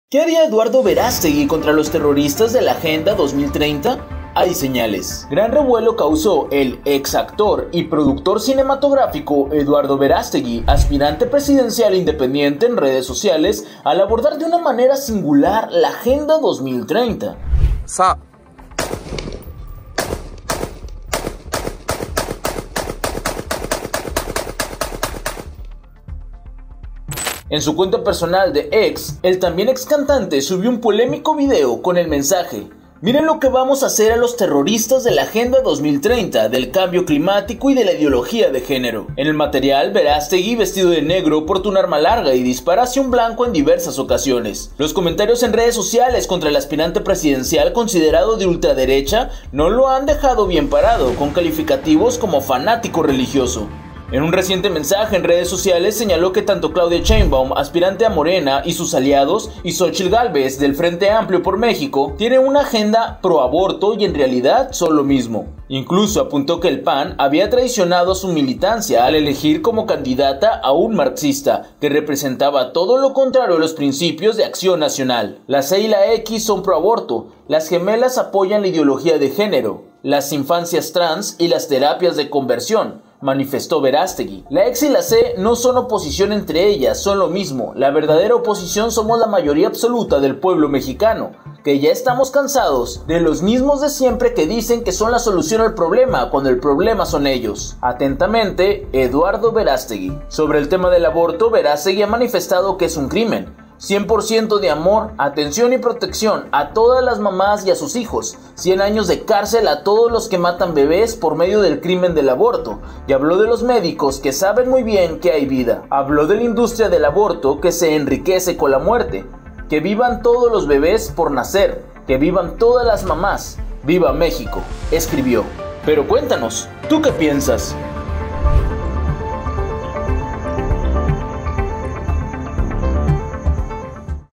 Eduardo Verástegui cuenta en redes sociales lo que haría contra los "terroristas" de la agenda 2030
En su cuenta personal de X, el también excantante subió un polémico video con el mensaje: Miren lo que le vamos a hacer a los terroristas de la agenda 2030, del cambio climático y de la ideología de género.